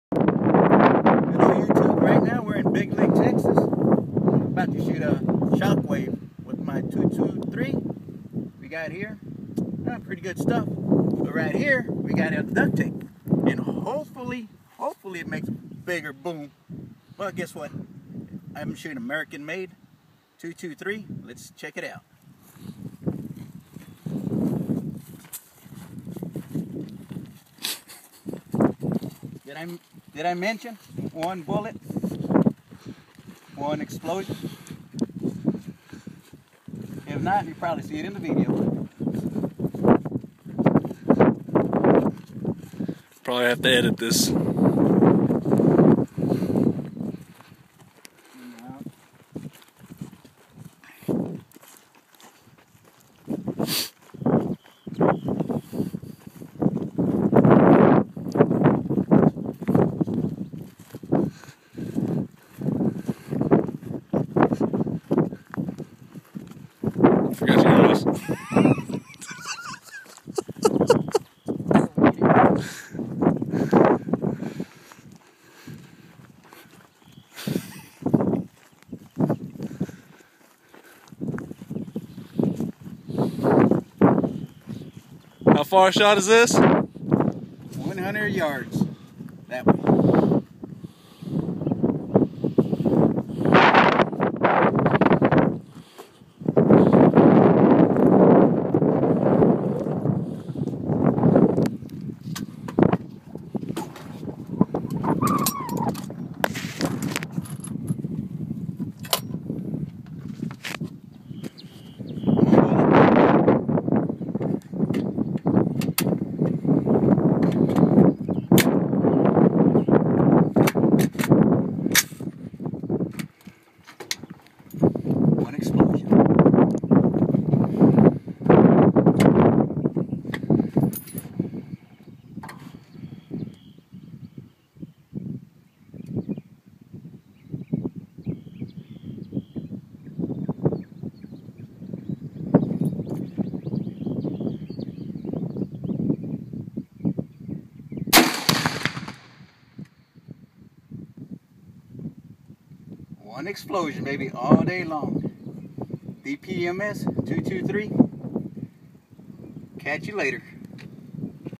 AR-15 vs. Exploding Target
My buddy explodes a Shockwave exploding target at 100 yds.